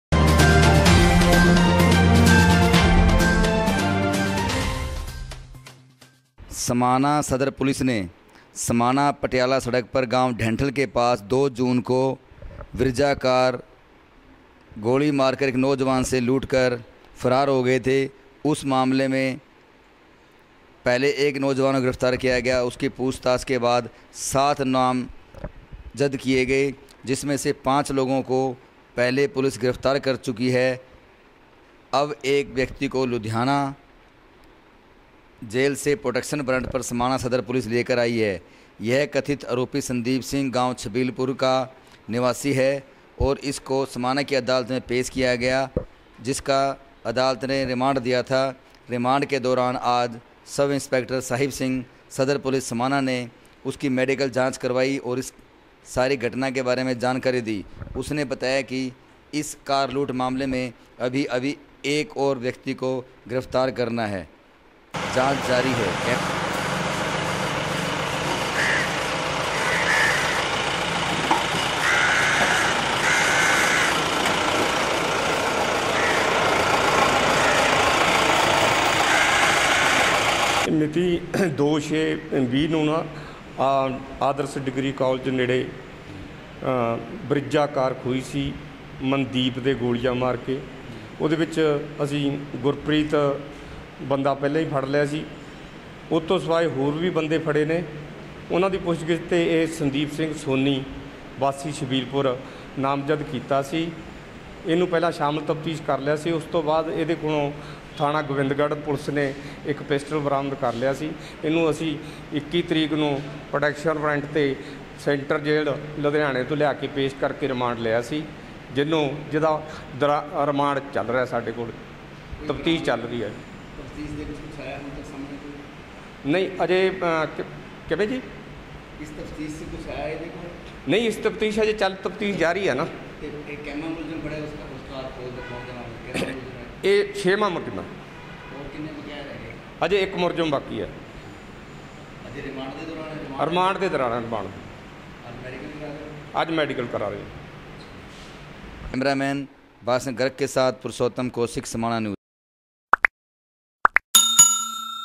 Brezza Car की लूट मामले में 6वा आरोपी गिरफ्तार प्रोडक्शन वारंट पर लुधियाना जेल से पुलिस रिमांड पर...
(News Updates - Ambala Jail से चलता है नेटवर्क Brezza Car की लूट मामले में 7 लोग नामजद 4 आरोपी 3 फरार...)

समाना- पटियाला सड़क पर 2 जून को गांव दुलार से मलकीत सिंह पटियाला जा रहा था कि गांव डेंटल के पास बृजा कार में लिफ्ट लेने वाले व्यक्ति ने उसको गोली मारकर कार लूट कर फरार हो गया था जिस पर पुलिस ने मामला दर्ज किया था एक आरोपी को कुछ दिन बाद गिरफ्तार कर लिया उससे पूछताछ में 7 लोगों के नाम और सामने आए थे जिनमें आज 6 व कथित आरोपी को भी लुधियाना जेल से प्रोडक्शन वारंट पर समाना सदर पुलिस ने लाकर समाना अदालत में पेश किया जहां उसका पुलिस रिमांड अदालत ने दिया है आज कथित आरोपी का समाना सदर पुलिस के सब इंस्पेक्टर साहिब सिंह की टीम ने मेडिकल जांच करवाई एवं इसके बारे में जानकारी दी उन्होंने बताया कि अभी इसका लूट मामले में एक आरोपी को गिरफ्तार करना है इस मुकदमे में 7 लोगों को नामजद किया गया था

Beyt-1 साहिब सिंह सब इंस्पेक्टर सदर पुलिस थाना समाना

पुरुषोत्तम कौशिक
Samana News